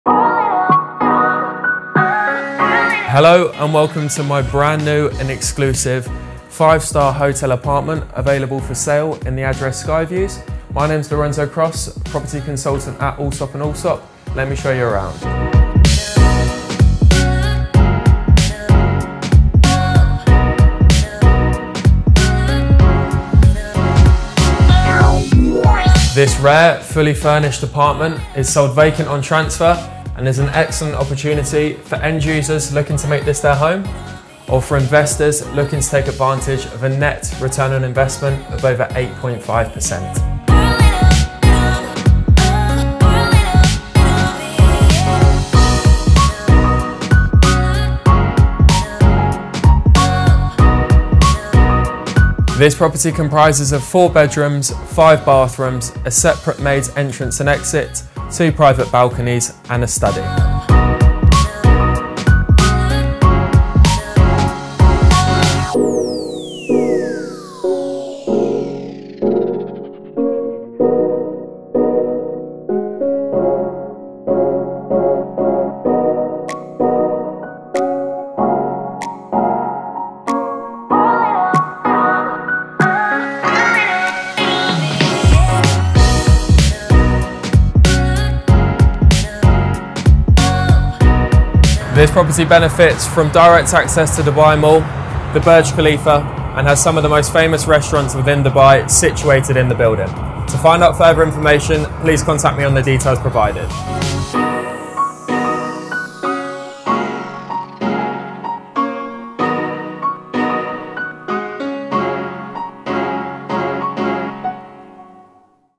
5 Star Hotel Living. Address Sky Views, Downtown Dubai
Live in the heart of the city!

This five-star fully serviced four-bedroom apartment is a part of The Address Sky View Hotel - one of Dubai's famous, high-end hotels. Living in this hotel will give you direct access to the worlds largest shopping mall, Dubai Mall.